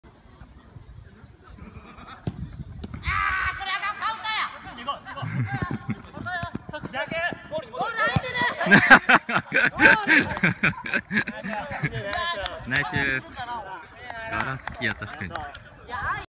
20161016万博ロッソフットサル オーリー→小倉 ガラ空きのゴールに流し込み
これはガラ空きですね